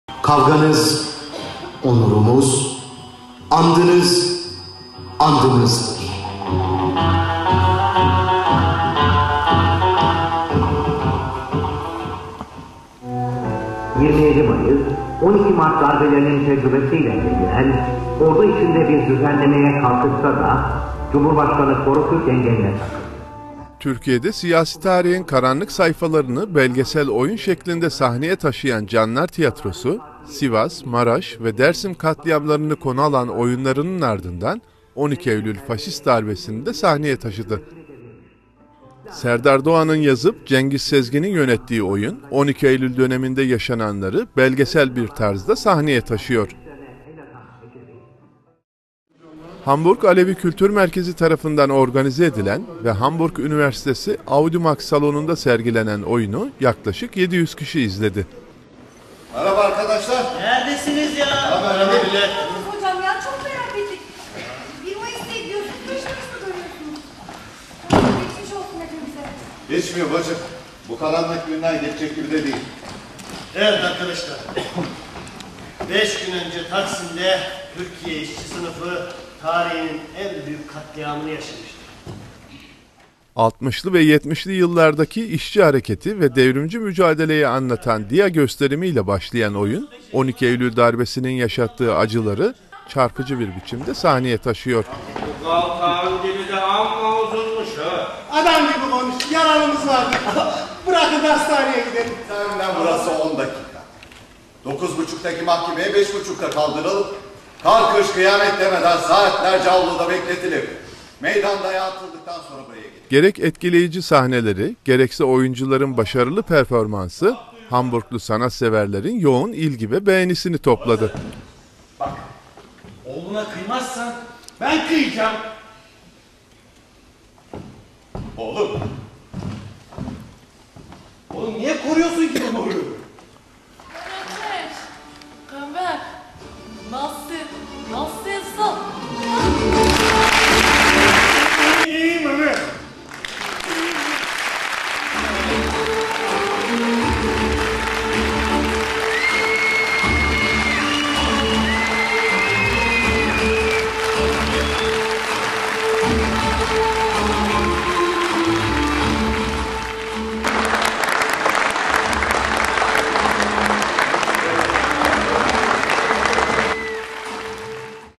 Canlar Tiyatrosu 12 Eylül Belgesel oyunu Hamburg gösterim haberi / Hayat TV Avrupa-2012
Canlar Tiyatrosu Hamburg gösterim haberi / Hayat TV Avrupa
12 Eylül Belgesel oyunu